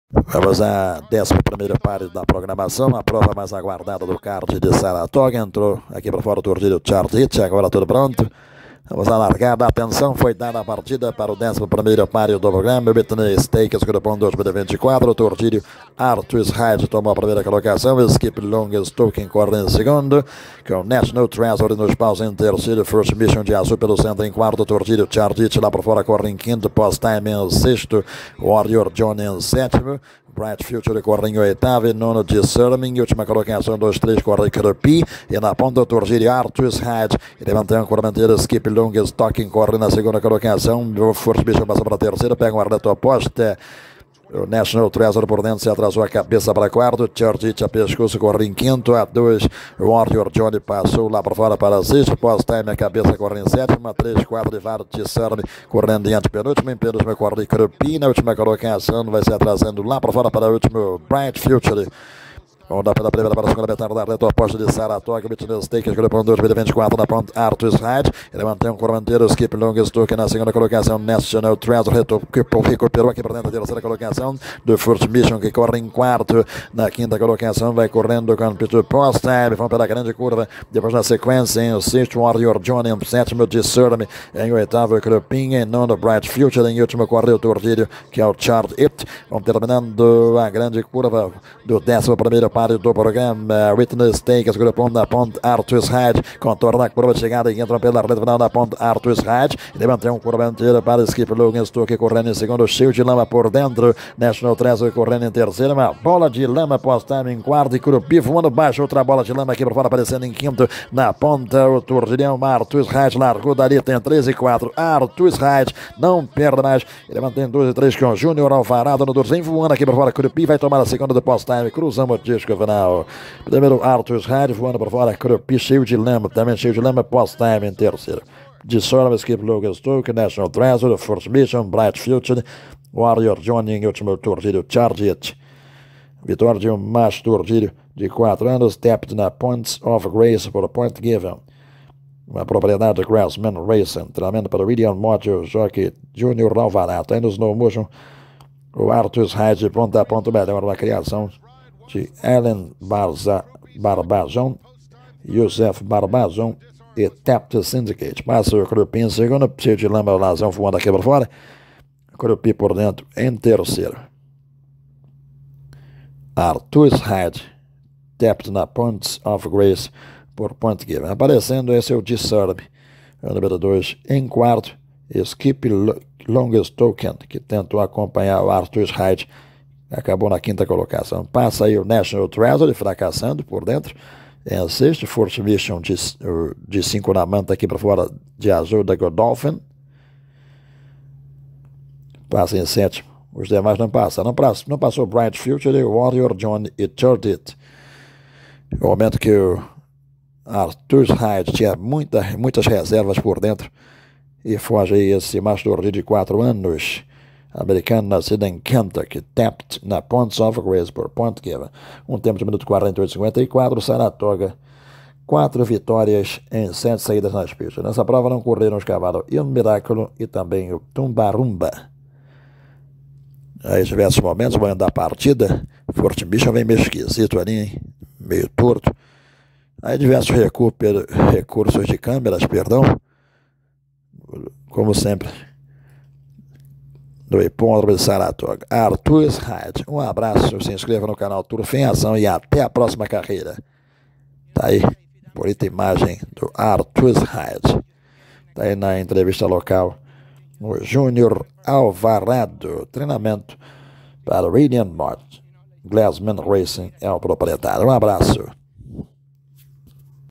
Whitney Stakes (G1) 2024
Track: Saratoga Race Course (BRZ)
Winner: Arthur's Ride (Tapit - Points Of Grace  by  Point Given)
Winner: Helen Barbazon, Joseph Barbazon, Tapit Syndicate
Owner: Glassman Racing
Trainer: William Mott
Jockey: Junior Alvarado